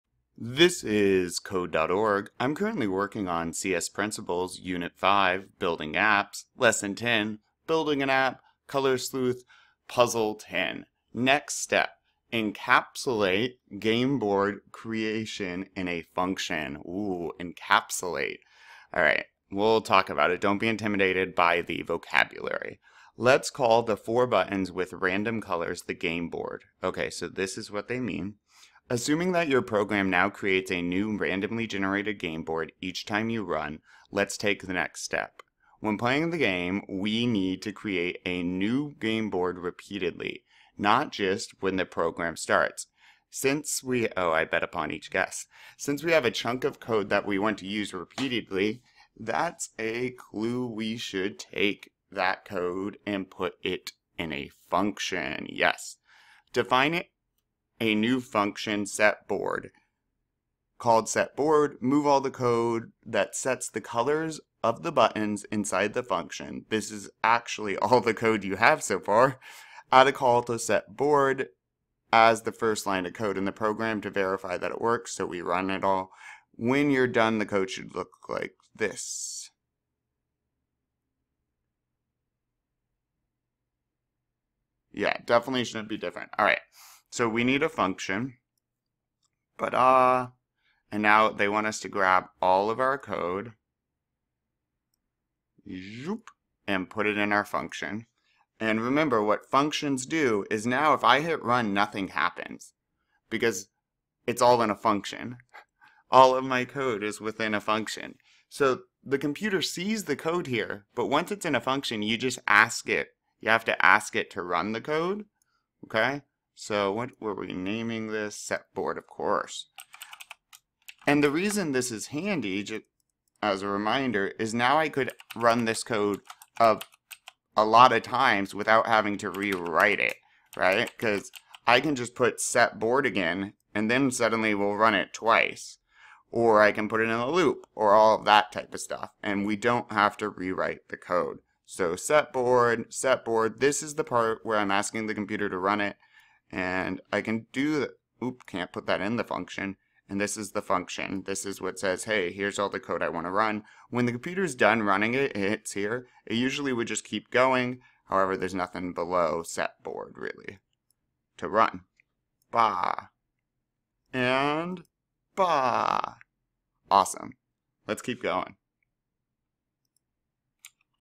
Building an App: Color Sleuth Lesson 10.10 Tutorial with Answers Code.org CS Principles
Building an App: Color Sleuth, lesson 10 from Building Apps, unit 5 of Code.org's C.S. Principles course, often used in AP Computer Science classrooms. Each part of the course challenges students to accomplish a new task while learning principles of computer science. In this video I walk through step-by-step how to get the right answer for this puzzle. Tutorials for other lessons is linked below.